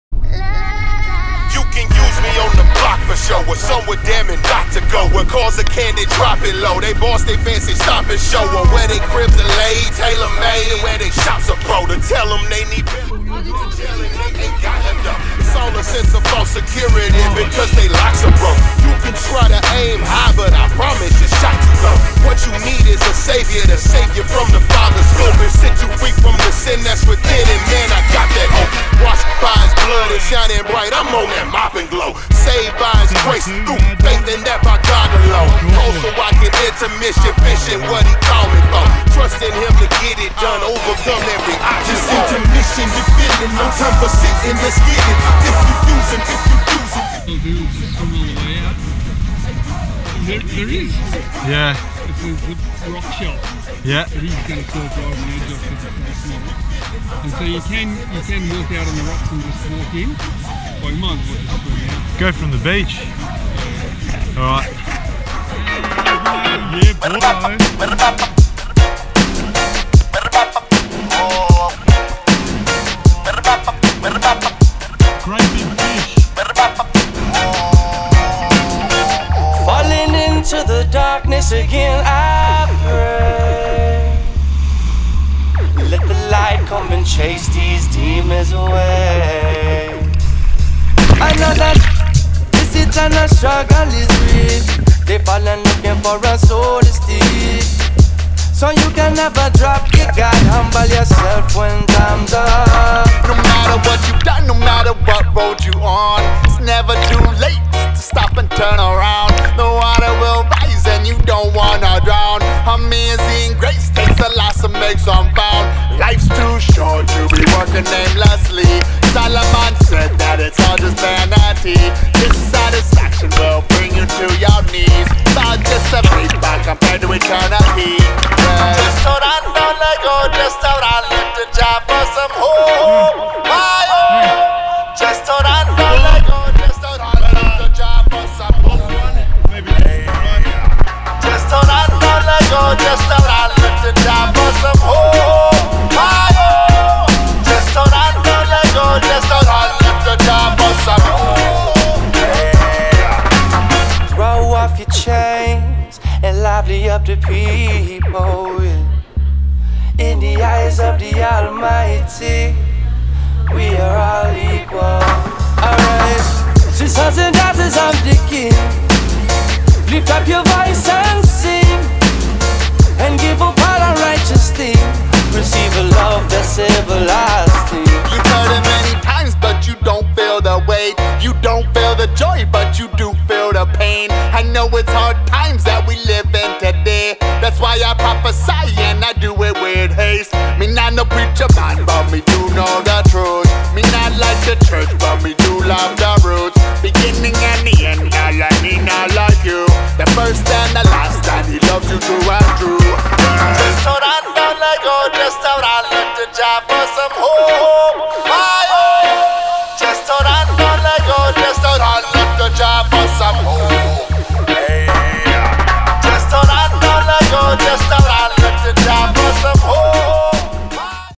Abalone Diving Catch And Cook Near Sydney NSW Spearfishing
We recently went to a local scout hall to camp by the beach near Newcastle. We we went diving and found a honeypot of abalone. The limits are 2 per person which was fine enough for me. Abalone is one of my favourite seafoods. The Marinate was sizzling Honey, Soy sauce and olive oil. A perfect combination